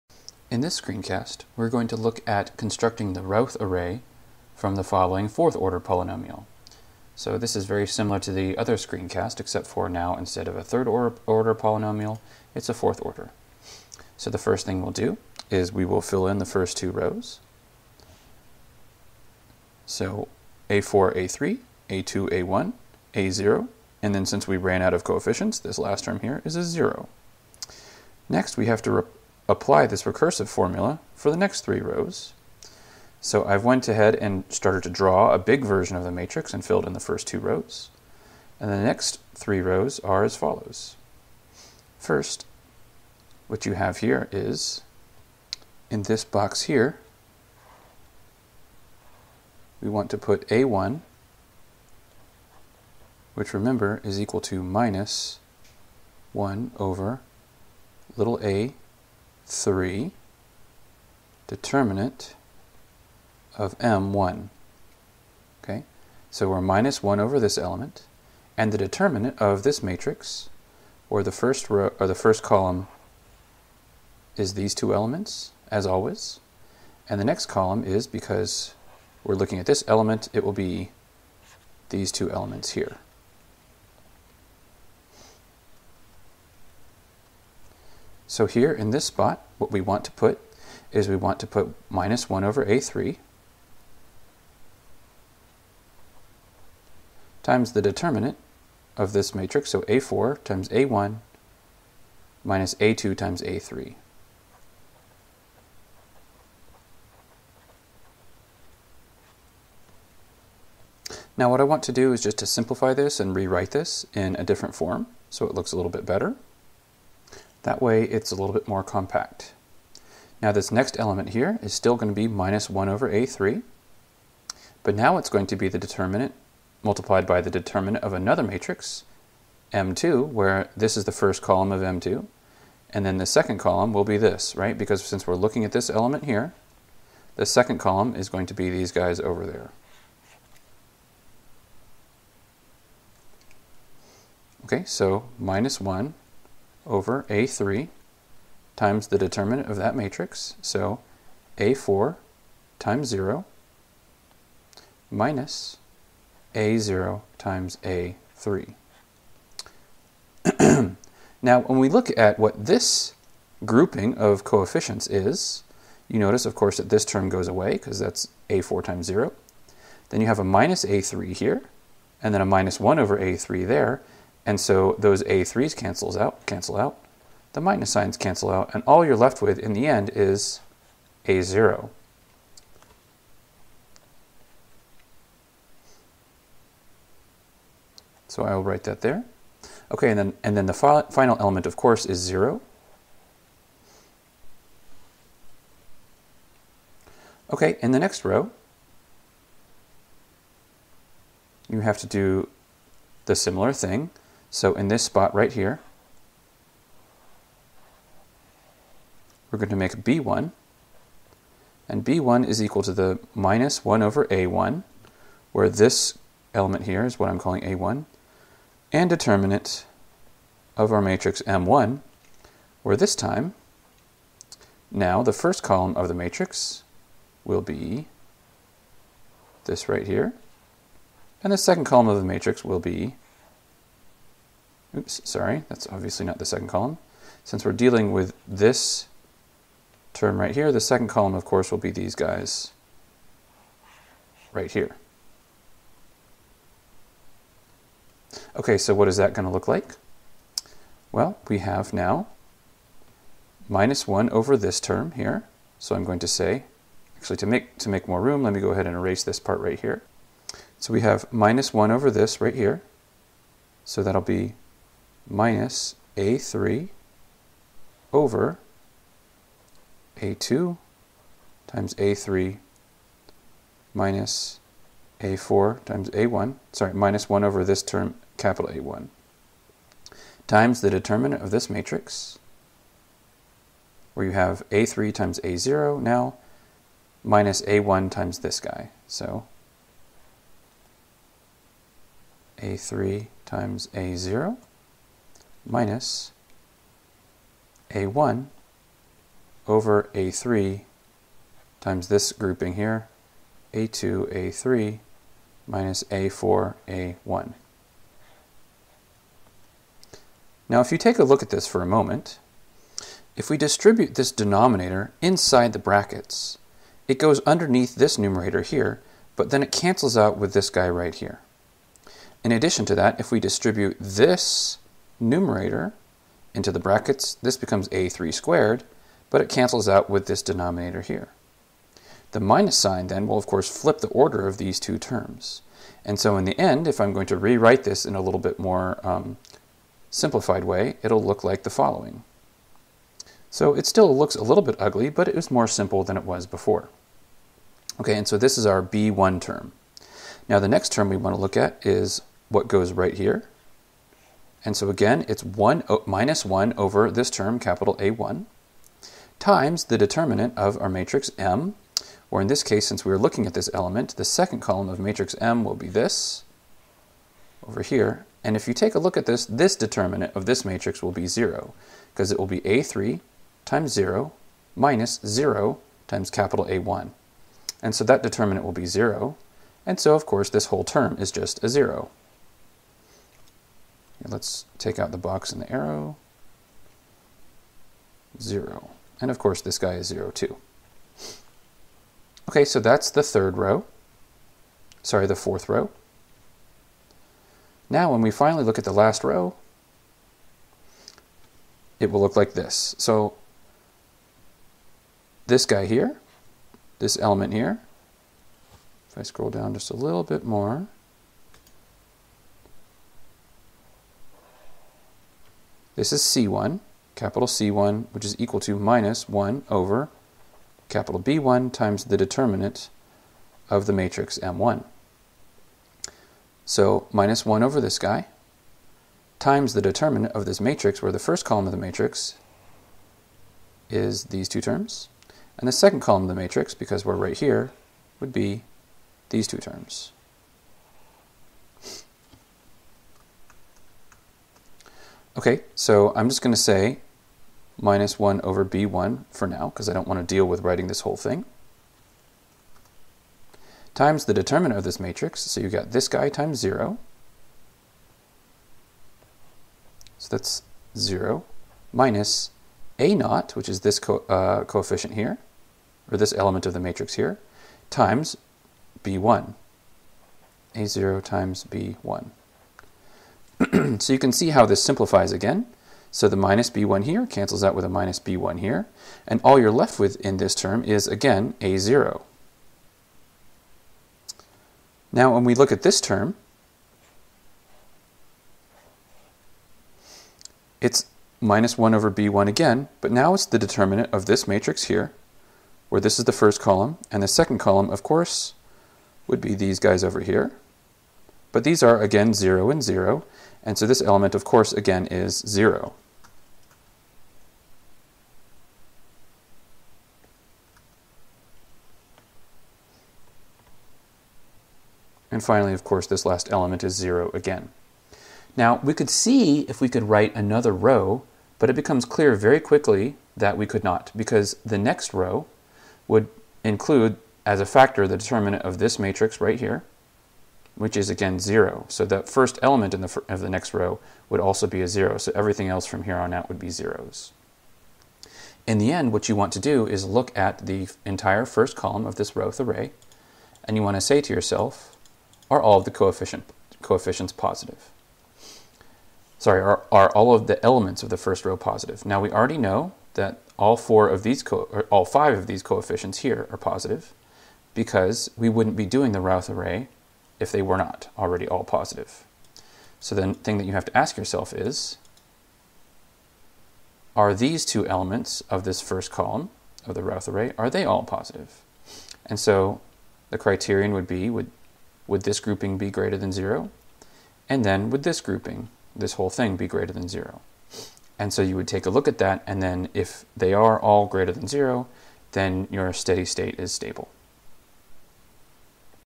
Routh 4th order example
In this screencast, we take a look at a fourth-order example of using the Routh array.  A screencast for ChE 435 (Process control) in the Chemical and Biomolecular Engineering department at NC State University.